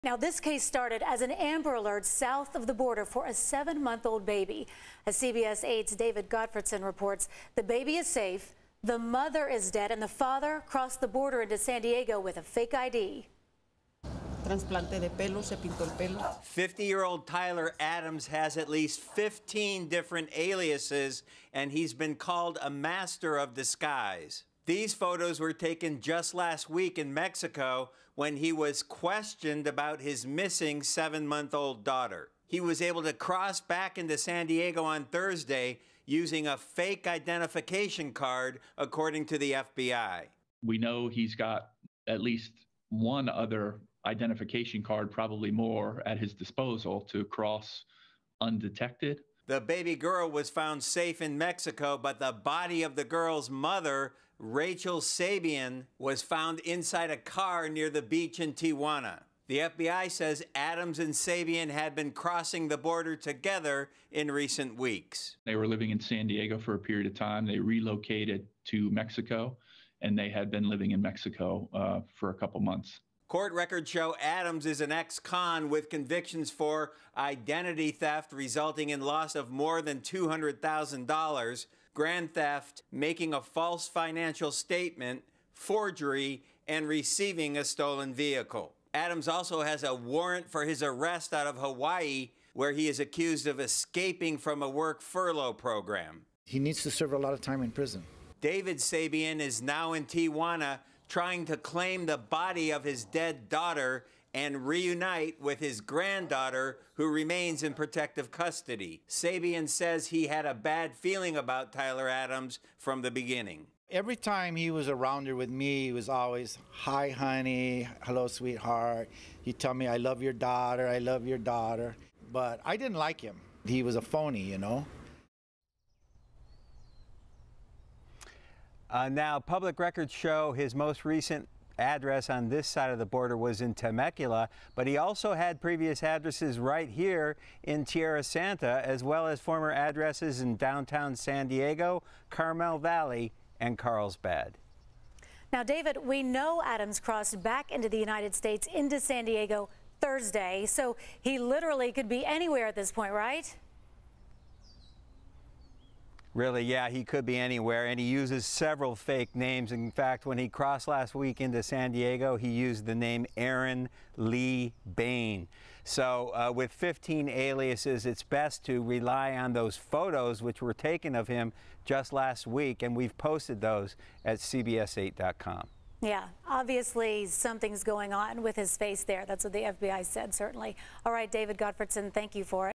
FBI searching for man after San Diego woman found dead in Mexico
The FBI is asking the public to help them find a man wanted for questioning after his girlfriend disappeared and was found dead in Mexico.

Authorities are searching for Tyler Adams and an FBI agent told CBS 8 that Adams is a master of disguise and he's dangerous.

Authorities found the body of his American girlfriend, Racquel Sabean, in a car in Mexico May 31. Adams was last seen crossing the border, using a fake ID, at the San Ysidro Port of Entry into San Diego Thursday afternoon.